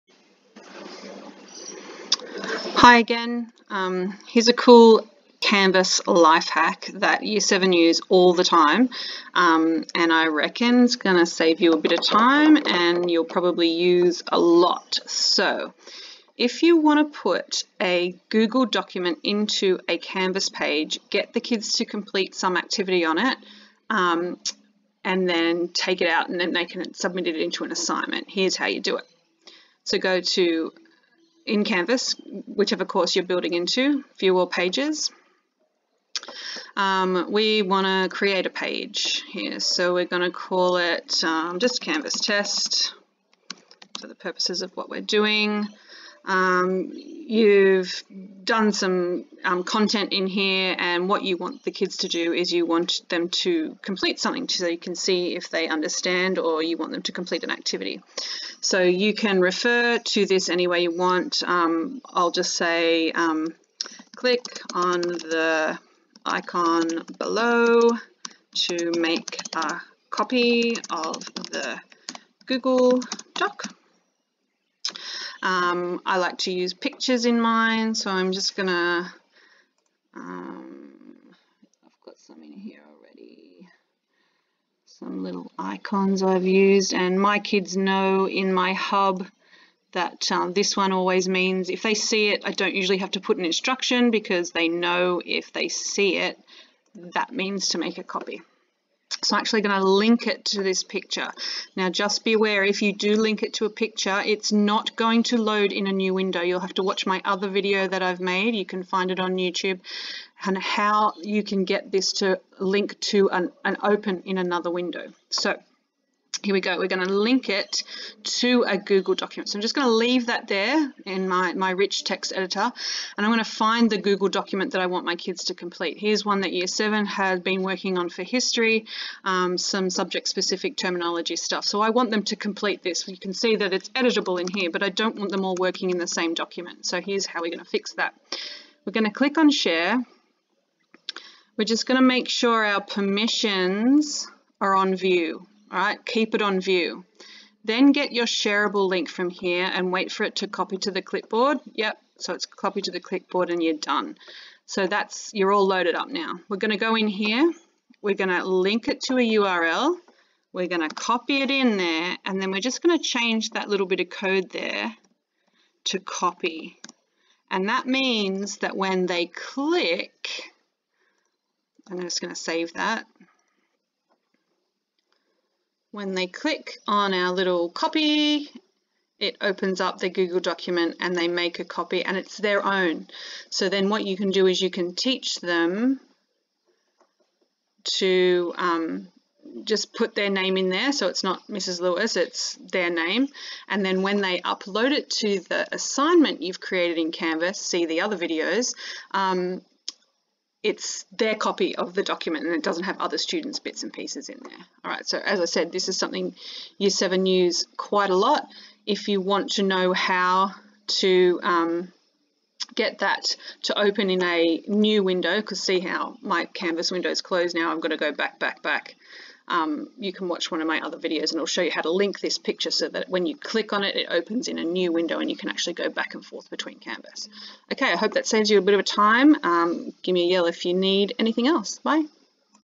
Linking to a google doc copy
Teacher Perspective: How to link a Google doc so it automatically makes a copy for students to complete an activity. They can then upload this to your Canvas assignment as formative assessment.